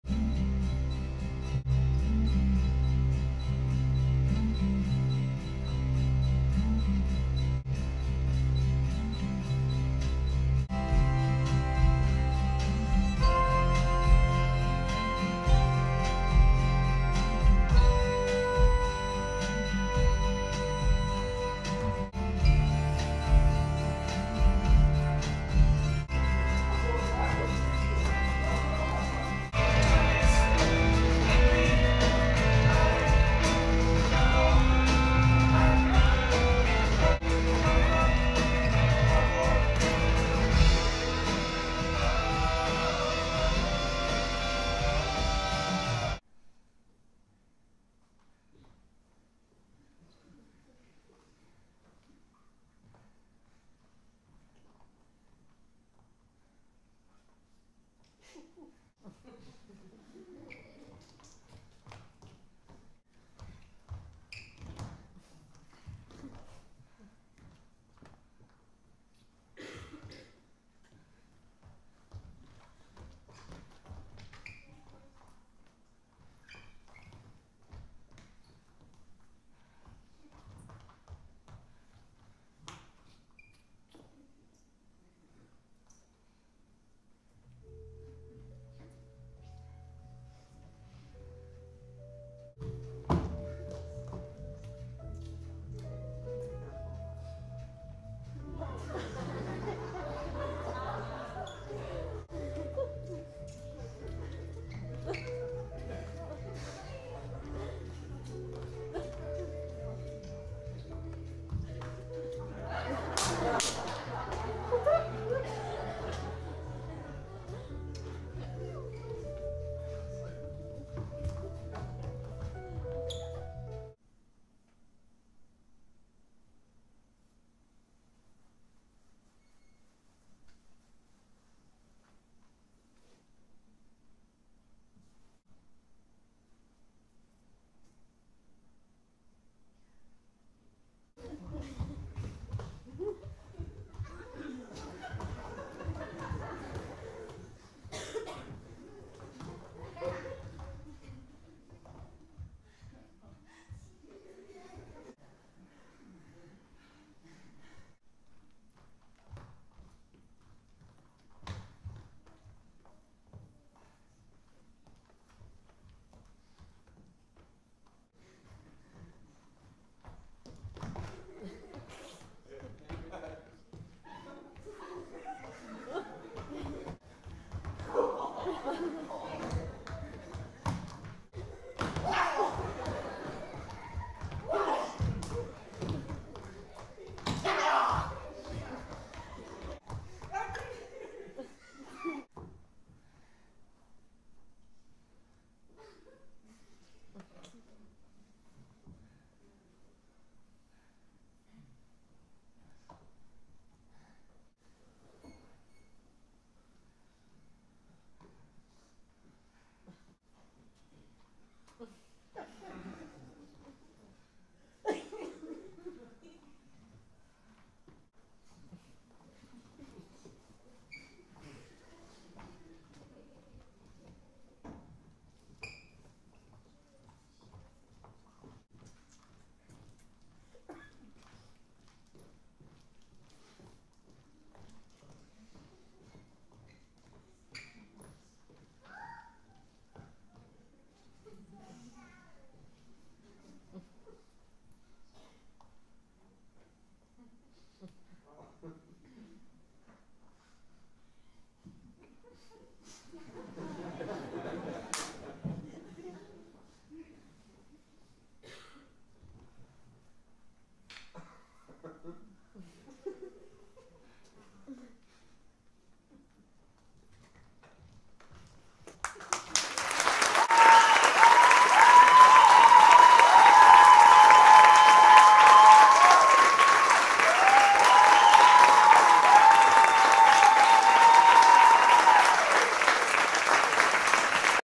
Lekker handig, from Hilde Elbers and Jasper Dzuki jelen
This is a family production made by Hilde Elbers and Jasper dzuki Jelen, together with Meekers (now Maas) and Dansateliers amsterdam.
Dancers: Hansel nezza, Rozemarijn de neve, Melanie wirz